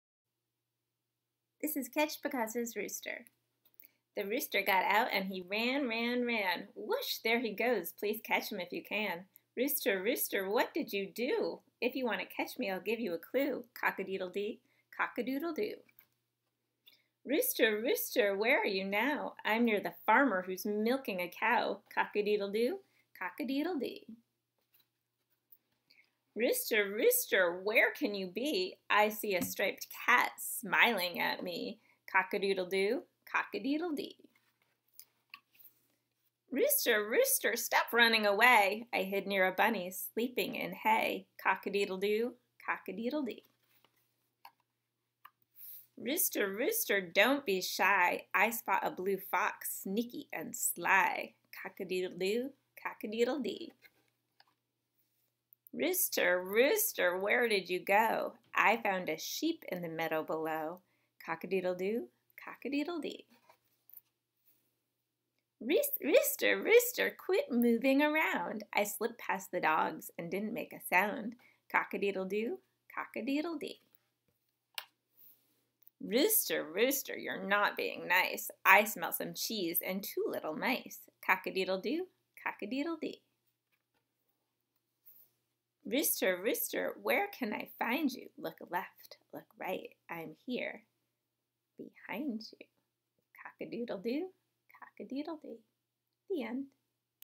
Catch Picasso's Rooster
Author Amy Guglielmo reads Catch Picasso's Rooster